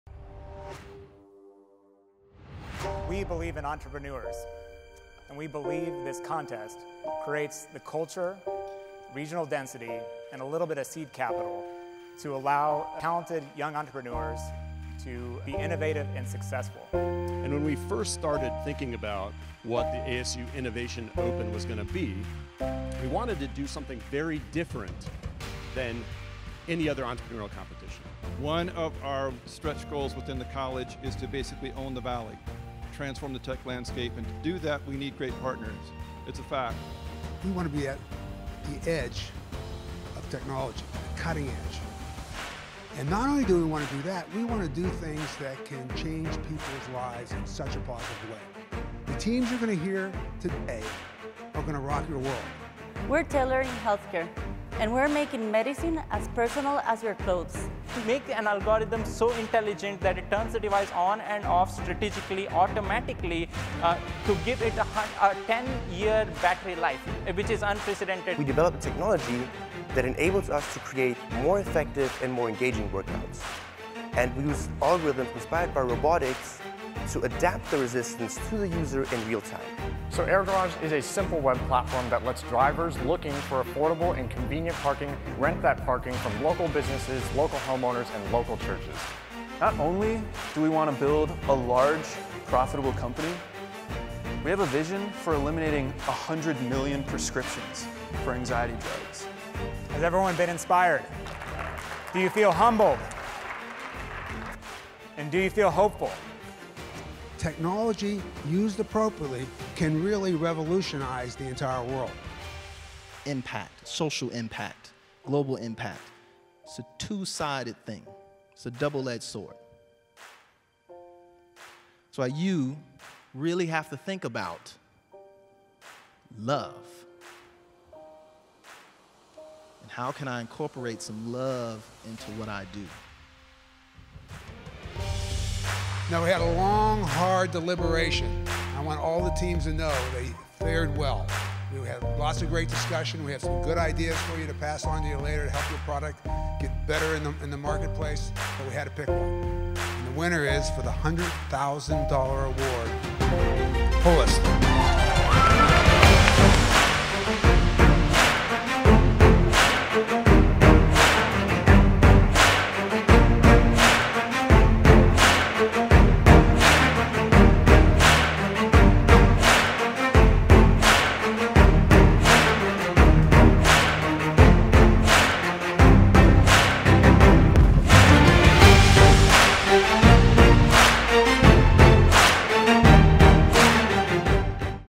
ASU Innovation Open
We are proud to sponsor ASU Innovation Open, an entrepreneurial competition that spans Phoenix to enable students to explore cutting edge technology that changes peoples lives for the better.